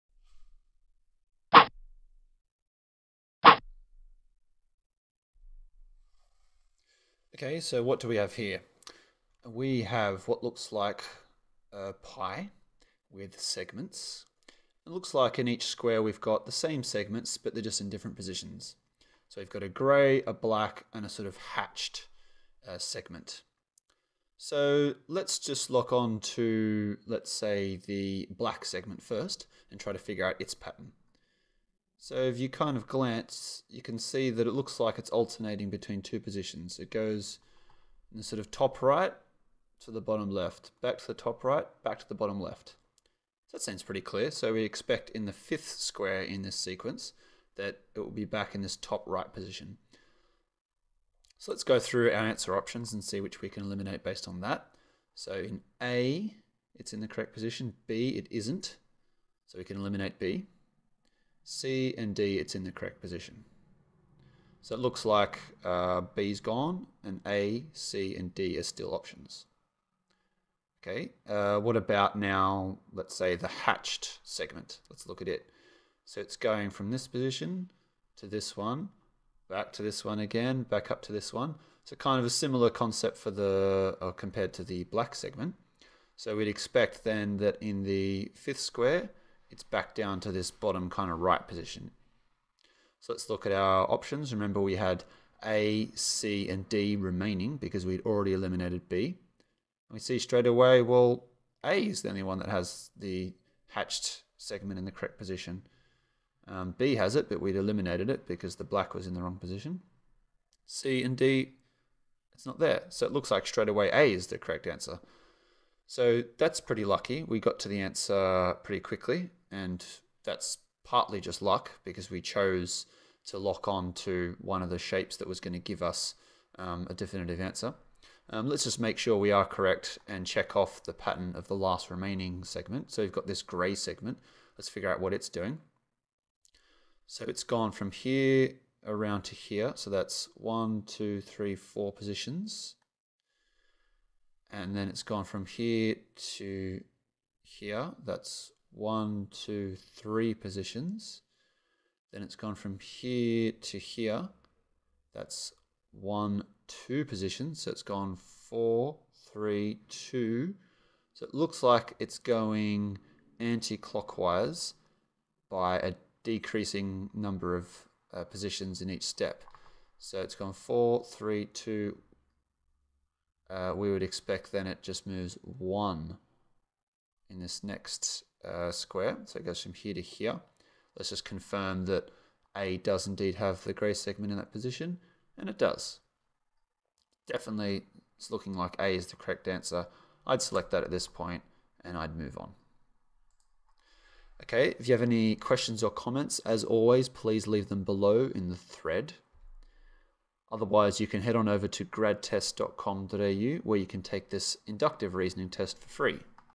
Free Inductive Reasoning Test 3 Question 4
This is the answer to the 4th question in the free inductive reasoning test on gradtests.com. Take this inductive reasoning practice test under timed test conditions right now:

Download a free help guide covering loads of tips and tricks for excelling at psychometric tests here: